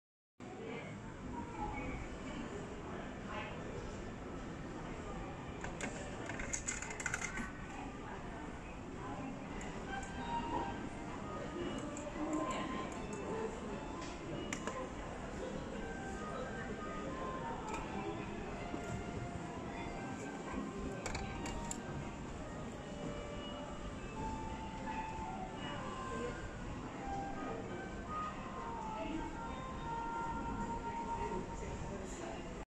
Try My Luck On Mini Fluffy Mascot Claw Machine
Lucky grab? Only in Japan.